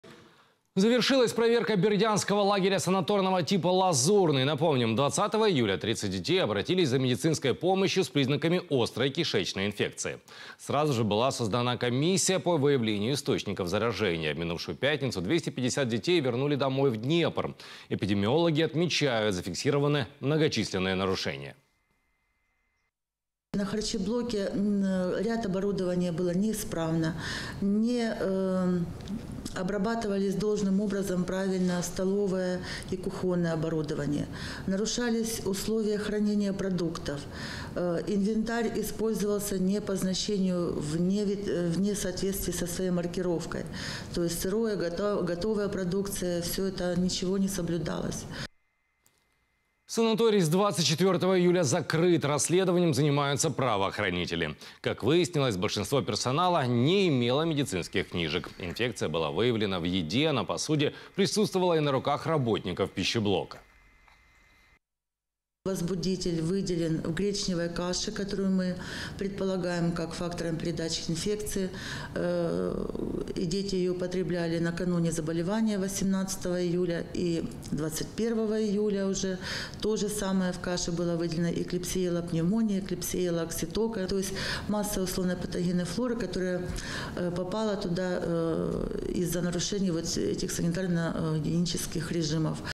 Закончилась проверка бердянского санатория «Лазурный»
Закончилась проверка бердянского санатория «Лазурный». Напомним, двадцатого июля тридцать детей обратились за медицинской помощью с признаками острой кишечной инфекции.  НАЧАЛО ВМЗ Сразу же   были созданы комиссии по выявлению источников заражения.  В минувшую пятницу 250 детей вернули домой в Днепр. Это дети в возрасте от 7 до 15 лет. Эпидемиологи отмечают, что были массовые нарушения.